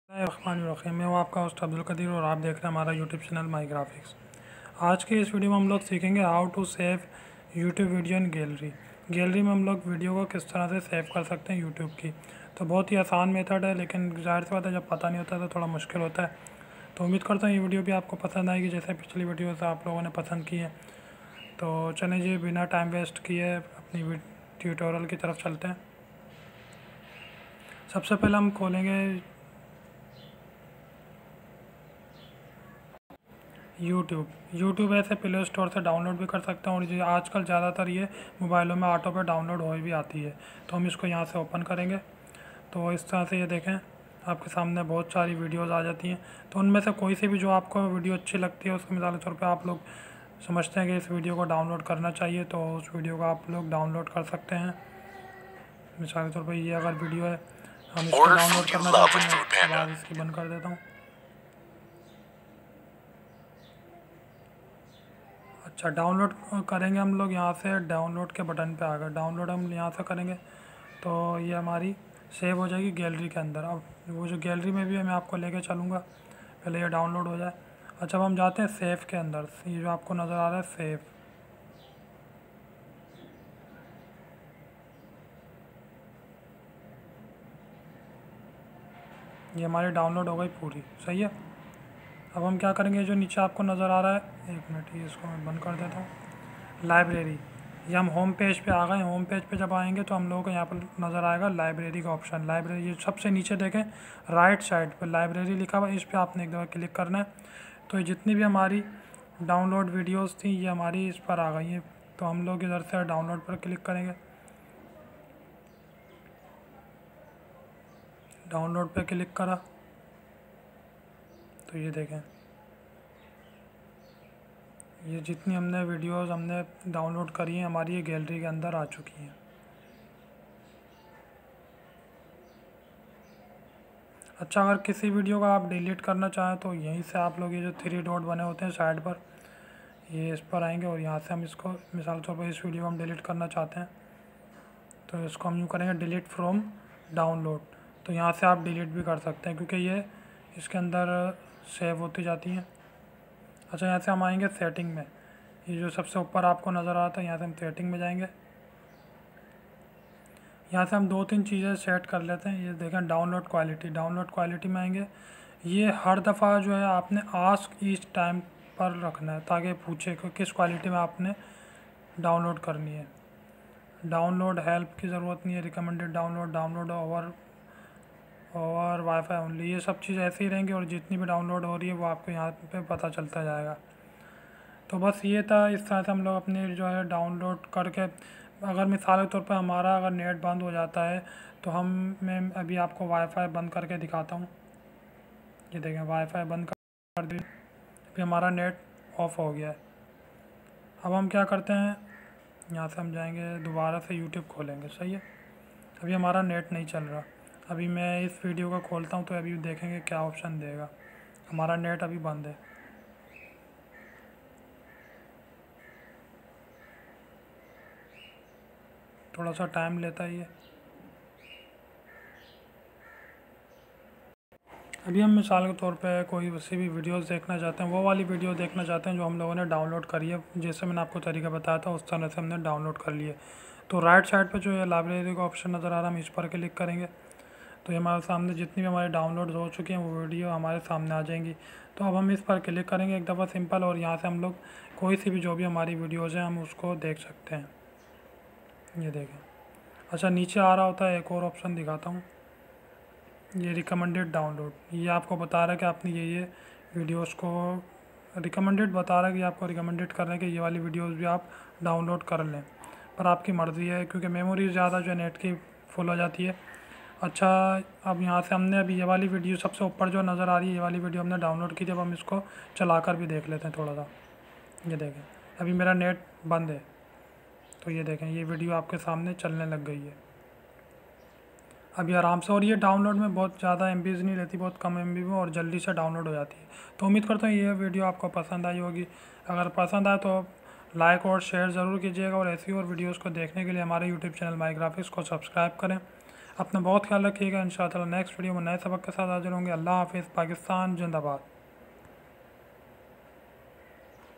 how to save youtube video in gallery in Urdu by mygraphics
salam

in this video we have learned how to save youtube video in gallery in Urdu by mygraphics